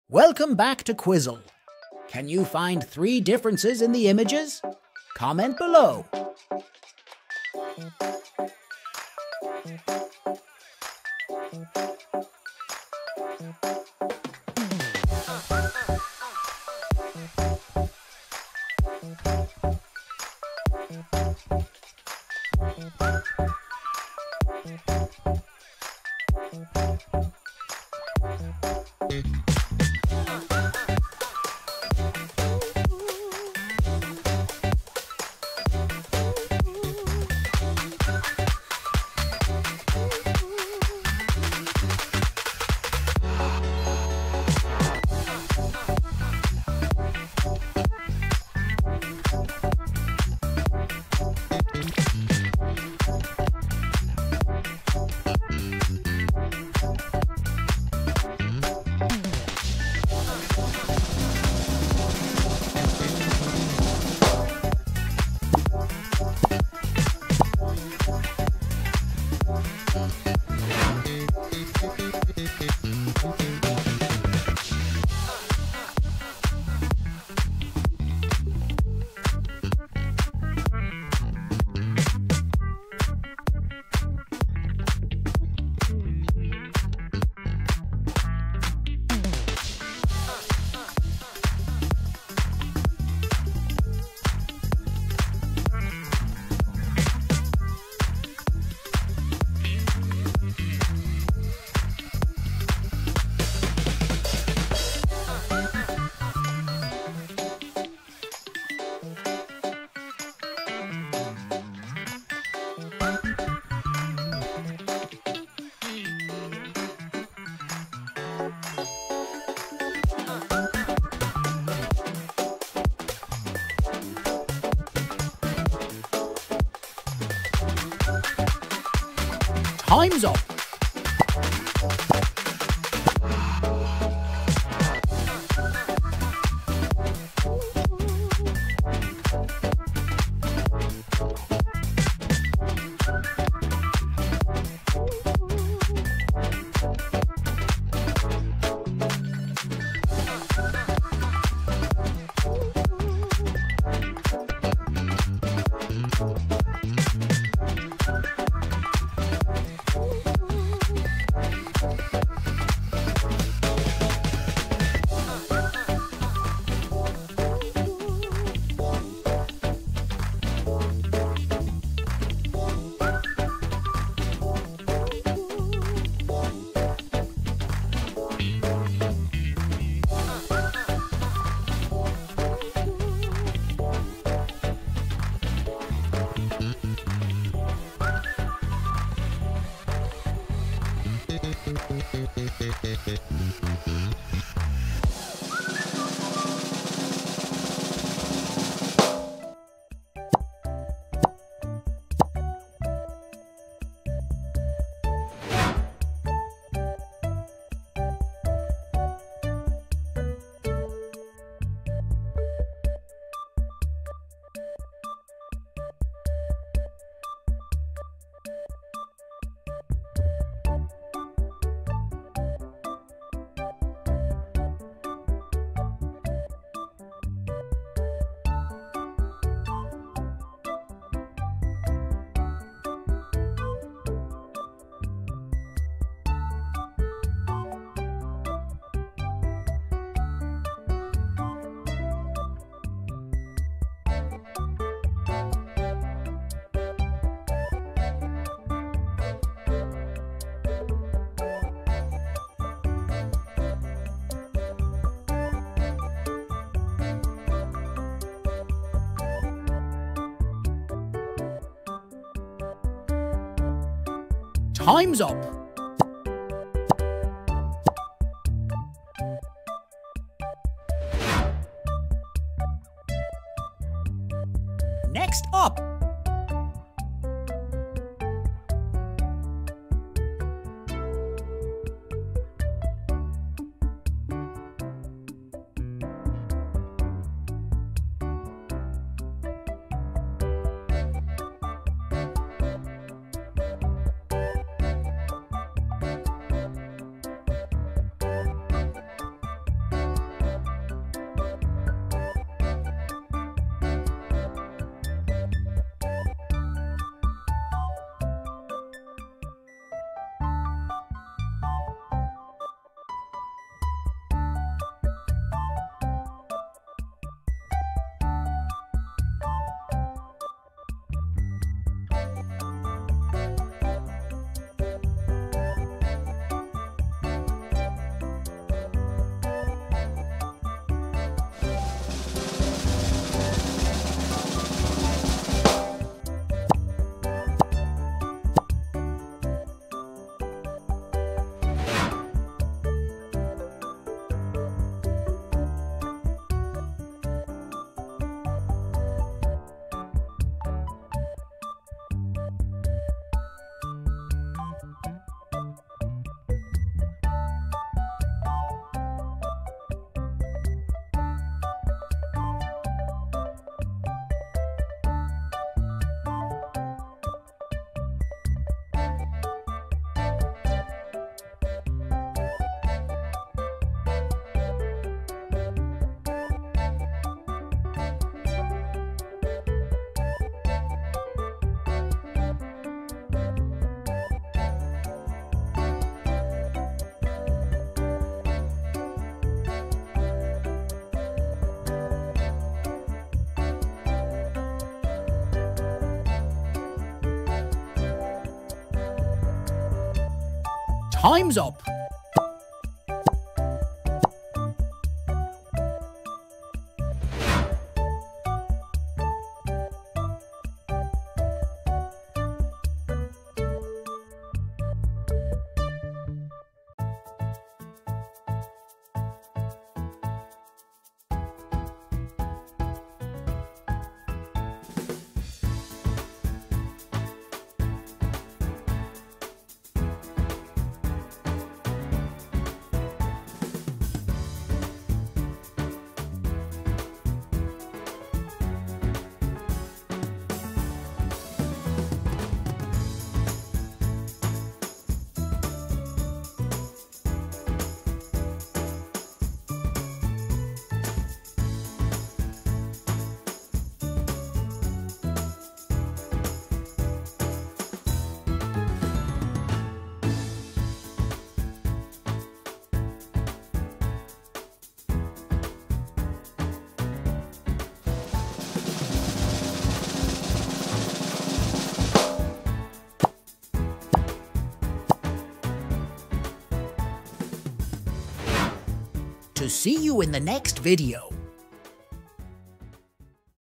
Spot The Difference Easy to Hard | Boost Your Oservation Skills #50 [Find The Difference]
Welcome to our 'Spot The Difference' puzzle game series! Challenge yourself with a range of puzzles from easy to impossible levels. Can you find all the differences? Join the fun and sharpen your observation skills today!"

Explore Puzzle Games on Amazon